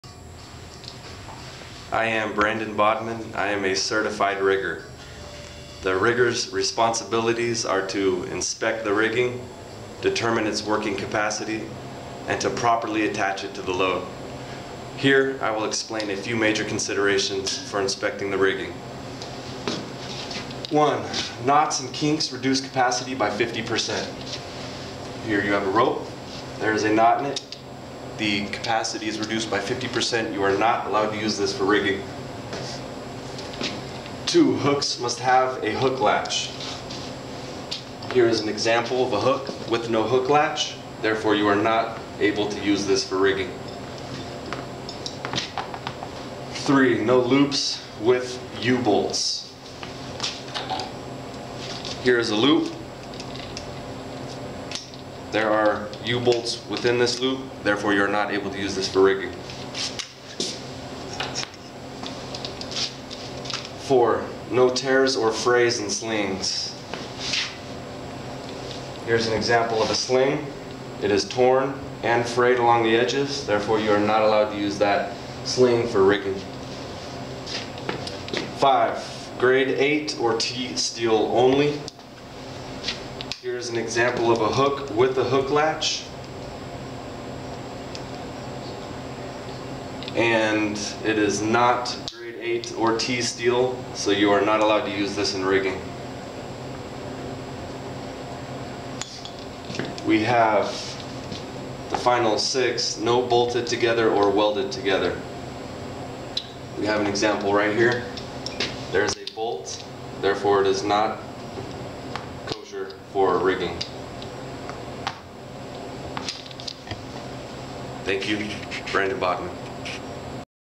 Certified Rigger Brandon Bodman (1of5)
A professional rigger needs to consider these 5 major Inspections to determine if rigging is safe and at working capacity. Trained and certified at Deep Creek Construction School.